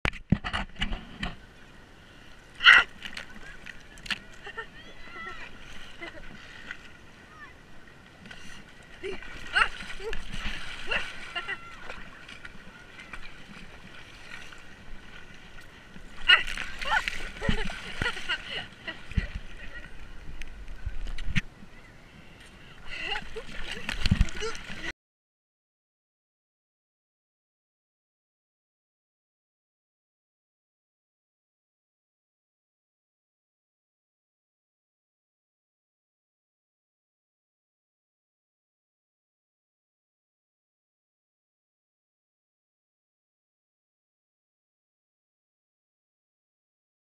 Water Gun
This is my water gun. Also known as "Max Liquidator".
Filmed at: Mills Beach
Filmed when: 31 December 2015
Filmed with: GoPro Hero 4 Action Video Camera (Silver Edition)